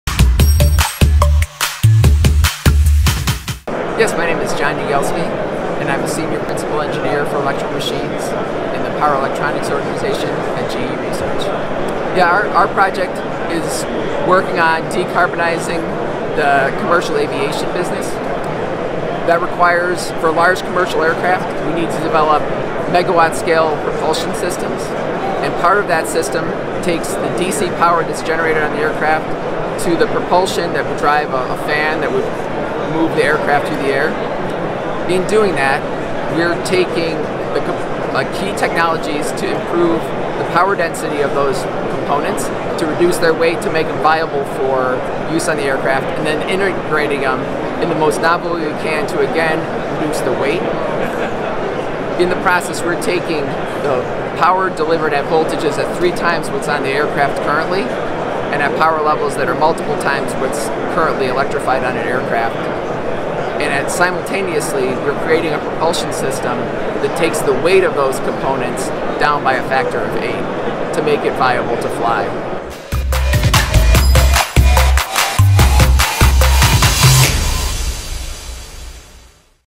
GE's eFLITES Project With ARPA-E to Advance Commercial Electric Flight
GE Research's John Yagielski, a Senior Principal Engineer and project leader for the Electric Flightworthy Lightweight Integrated Thermally Enhanced Powertrain System (eFLITES) project, at the ARPA-E Innovation Summit in Denver, Colorado this past Spring, discussing GE's efforts through ARPA-E's  Aviation-class Synergistically Cooled Electric-motors with iNtegrated Drives (ASCEND) program to create an electric propulsion system suitable for commercial aircraft.